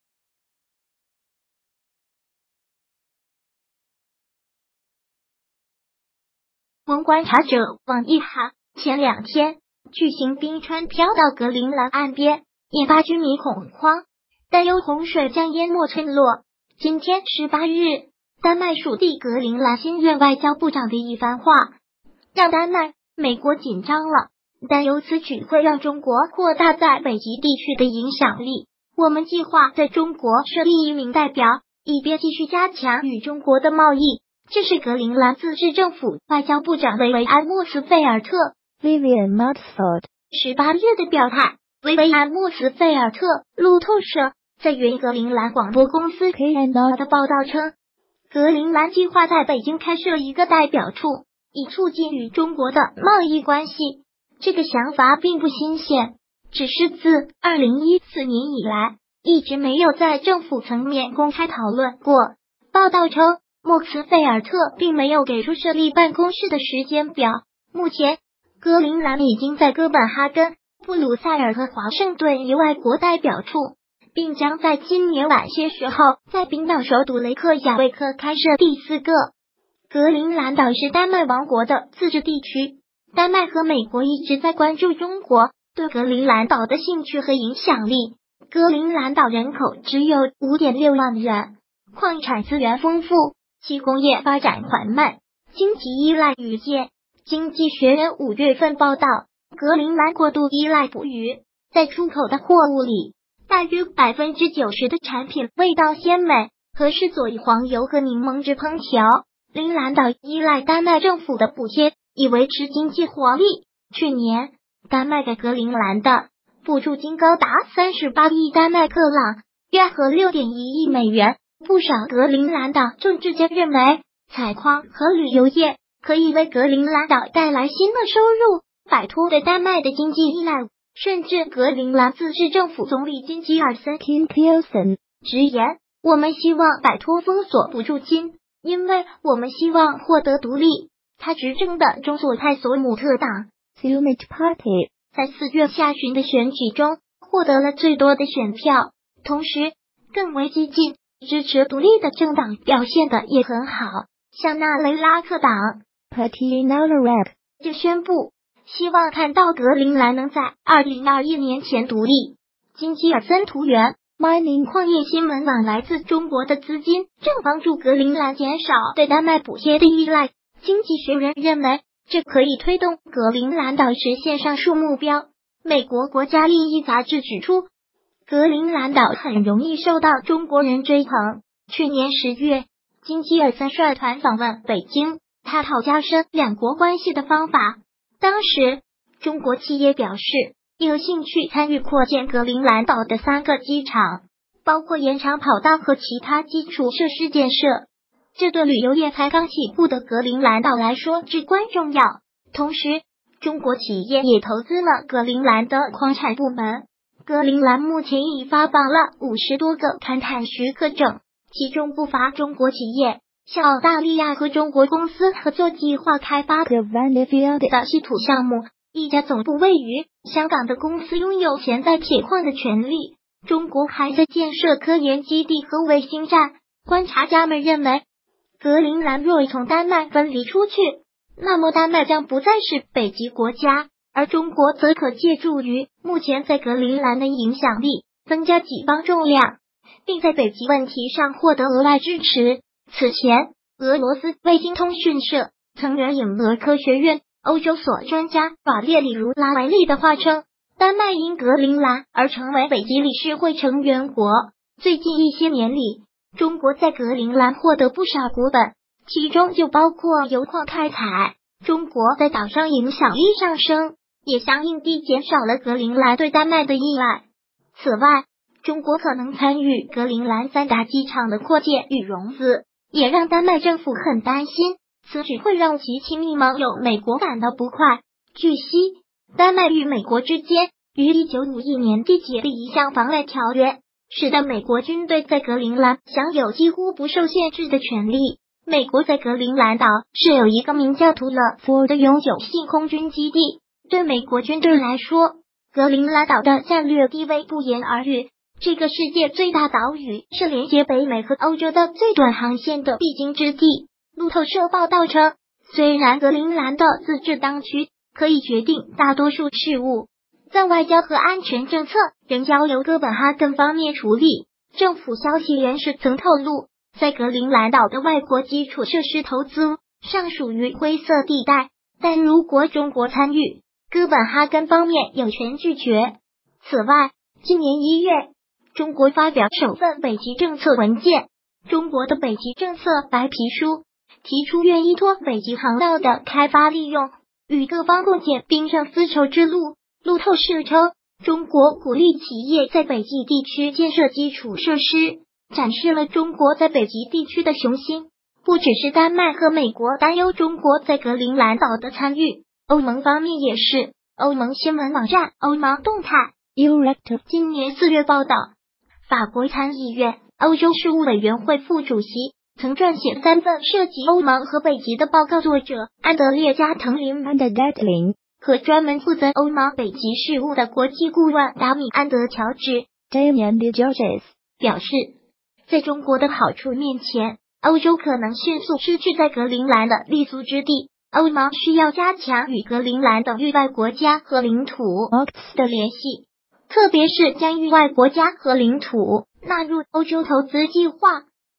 格陵兰岛这个与中国有关的计划，让美国和丹麦慌了
【文/观察者网 奕含】前两天，巨型冰川漂到格陵兰岸边，引发居民恐慌，担忧洪水将淹没村落。今天（18日），丹麦属地格陵兰新任外交部长的一番话，让丹麦、美国紧张了，担忧此举会让中国扩大在北极地区的影响力。“我们计划在中国设立一名代表，以便继续加强与中国的贸易。”这是格陵兰自治政府外交部长维维安·莫茨费尔特（Vivian Motzfeldt）18日的表态。维维安·莫茨费尔特路透社在援引格陵兰广播公司（KNR）的报道称，格陵兰计划在北京开设一个代表处，以促进与中国的贸易关系。这个想法并不新鲜，只是自2014年以来，一直没有在政府层面公开讨论过。报道称，莫茨费尔特并没有给出设立办公室的时间表。目前，格陵兰已经在哥本哈根、布鲁塞尔和华盛顿有外国代表处，并将在今年晚些时候在冰岛首度雷克雅未克开设第四个。格陵兰岛是丹麦王国的自治地区。丹麦和美国一直在关注中国对格陵兰岛的兴趣和影响力。格陵兰岛人口只有5.6万人，矿产资源丰富，其工业发展缓慢，经济依赖渔业。《经济学人》5月份报道，格陵兰过度依赖捕鱼。在出口的货物里，大约90%的产品味道鲜美，合适佐以黄油和柠檬汁烹调。陵兰岛依赖丹麦政府的补贴，以维持经济活